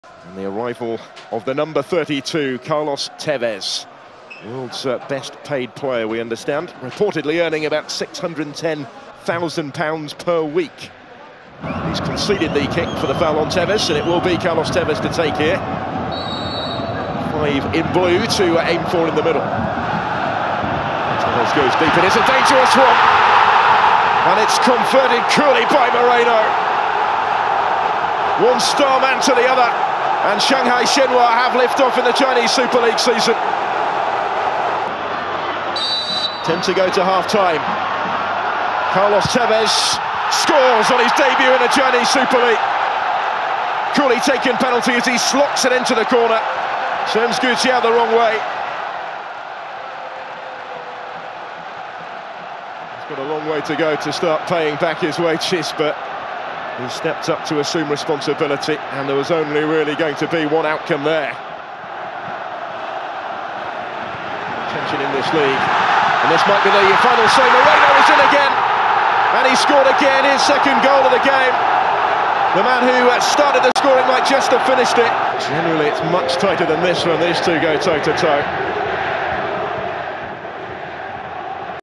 Carlos Tevez stars as Shanghai Shenhua win
Carlos Tevez scored on his Chinese Super league debut, as Shanghai Shenhua beat Jiangsu Suning 4–0. Tevez, the best-paid player in world football, also provided two assists.